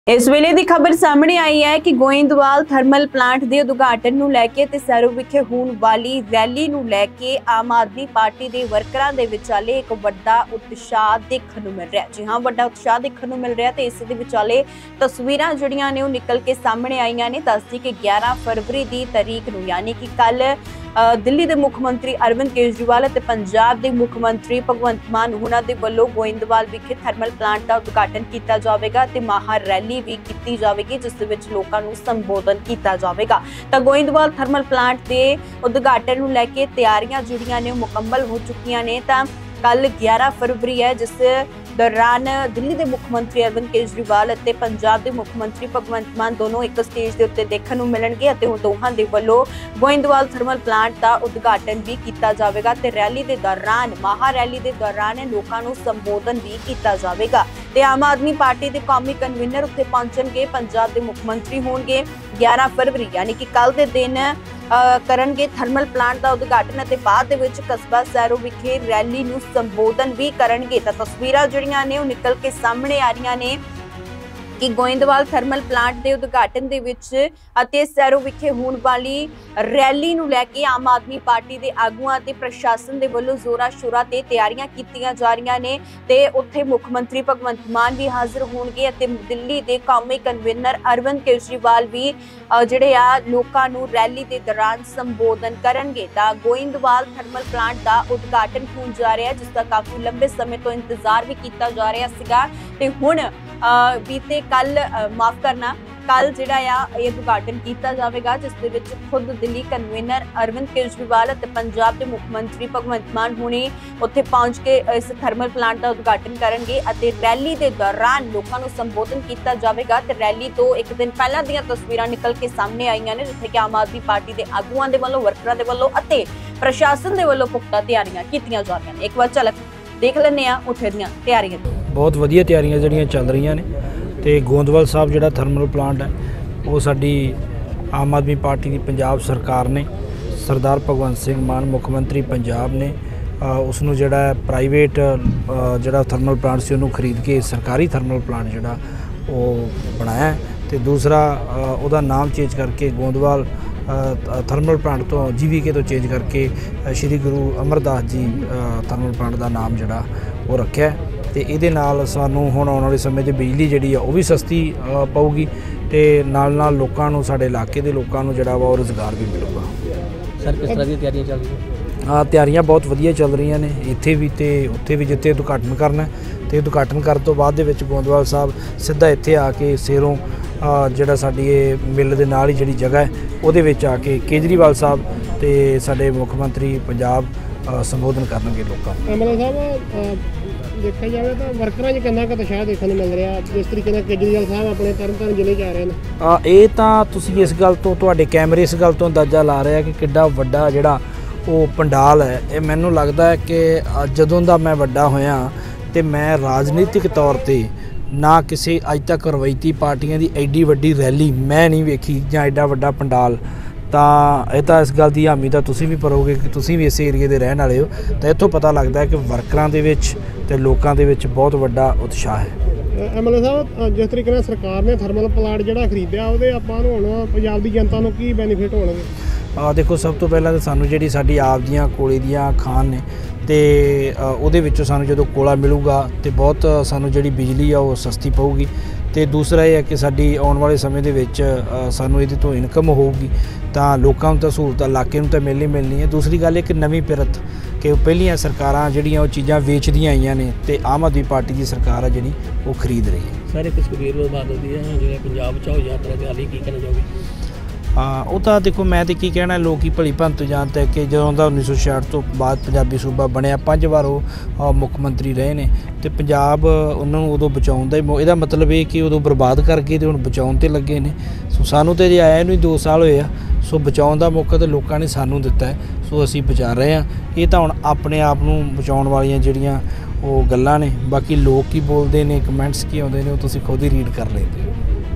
ਕੱਲ੍ਹ CM ਮਾਨ 'ਤੇ ਕੇਜਰੀਵਾਲ ਕਰਨਗੇ ਗੋਇੰਦਵਾਲ ਥਰਮਲ ਪਲਾਂਟ ਦਾ ਉਦਘਾਟਨ! ਰੈਲੀ ਨੂੰ ਲੈ ਆਪ ਆਗੂਆਂ ਵੱਲੋਂ ਕੀਤੀ ਤਿਆਰੀ
ਕੱਲ੍ਹ CM ਮਾਨ 'ਤੇ ਕੇਜਰੀਵਾਲ ਕਰਨਗੇ ਗੋਇੰਦਵਾਲ ਥਰਮਲ ਪਲਾਂਟ ਦਾ ਉਦਘਾਟਨ ! ਰੈਲੀ ਨੂੰ ਲੈ ਆਪ ਆਗੂਆਂ ਵੱਲੋਂ ਕੀਤੀ ਜਾ ਰਹੀਆਂ ਤਿਆਰੀਆਂ